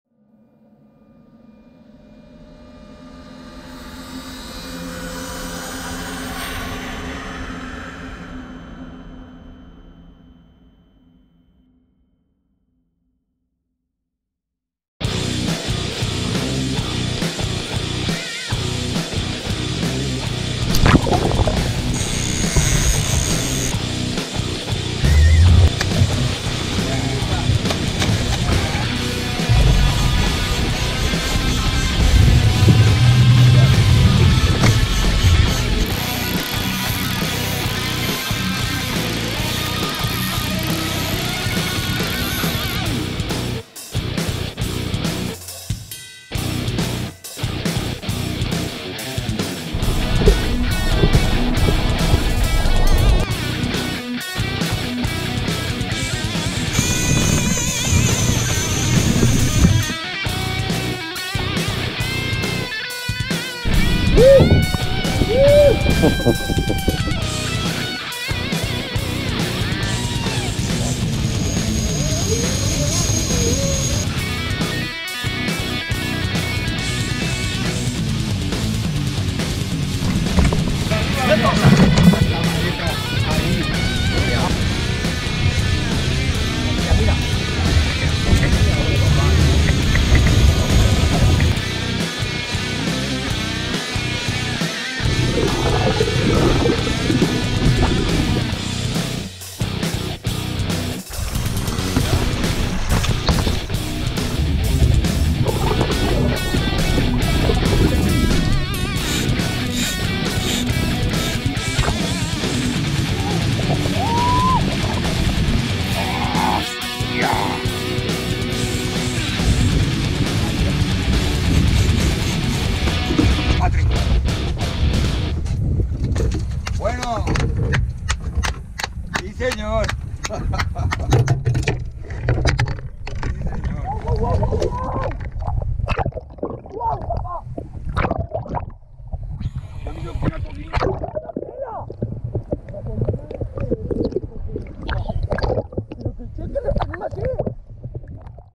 Atún Rojo Temporada 2019, trailer. Bluefin Tuna Mediterranean
Pesca de Atún Rojo desde embarcación en Valencia temporada 2019.
En este vídeo encontraréis un avance de lo que ha sido la pesca del Big Game esta temporada, que ha estado llena de emociones y sensaciones inolvidables.
Bluefin tuna fishing in the Mediterranean.
Trailer tuna fishing 2019.

Gracias por vuestro tiempo amigos, buena pesca a todos!¡
Tight lines¡!
Thanks¡!